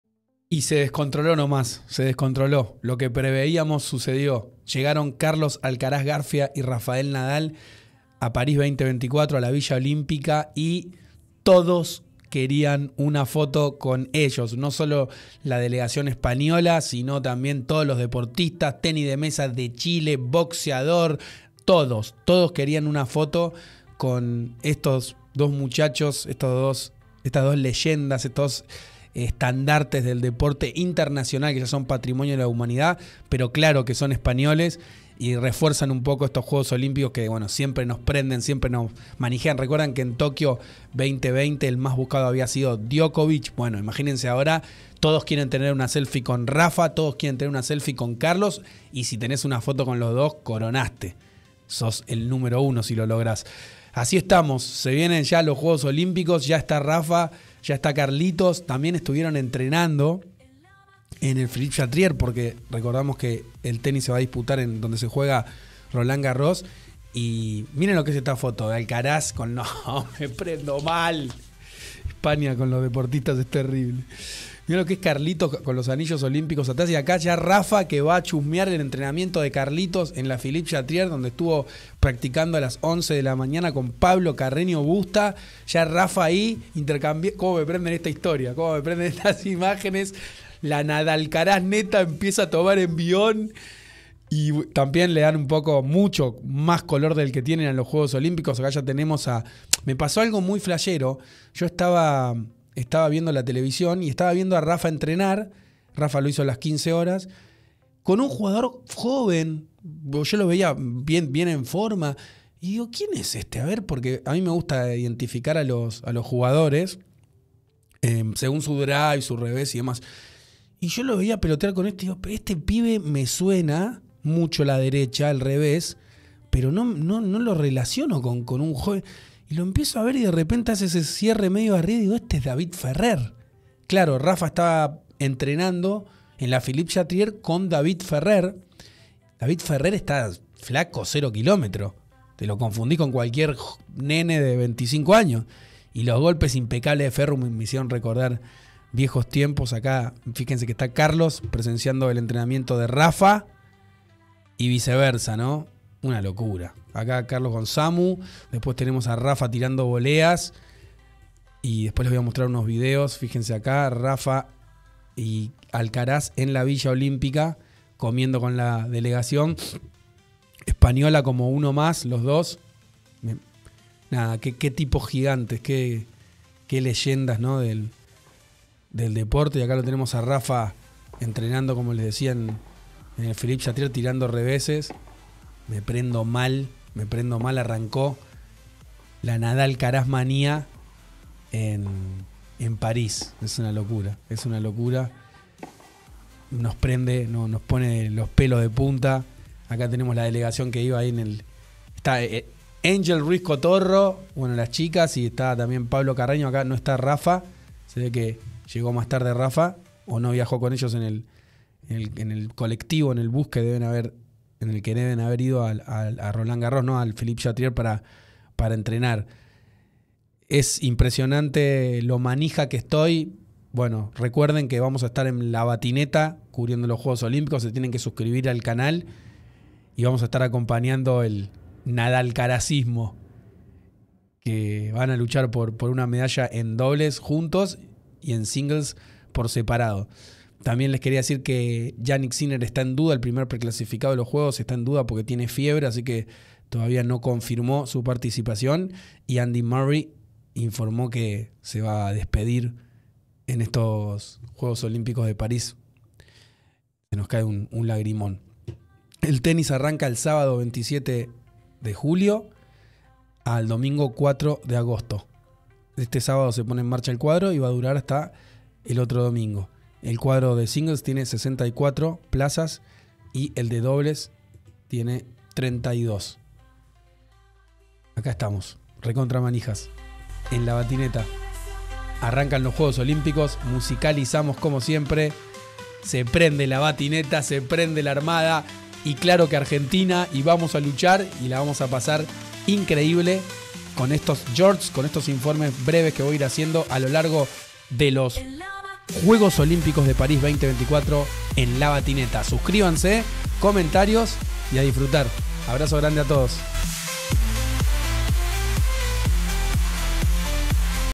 Nadal y Alcaraz ya están en los Juegos Olímpicos de Paris 2024 y revolucionan la villa
Rafael Nadal y Carlos Alcaraz NADALCARAZ llegaron a Paris y se revolucionó la Villa Olímpica. Todos quieren una foto con Carlitos Alcaraz y con Rafael Nadal.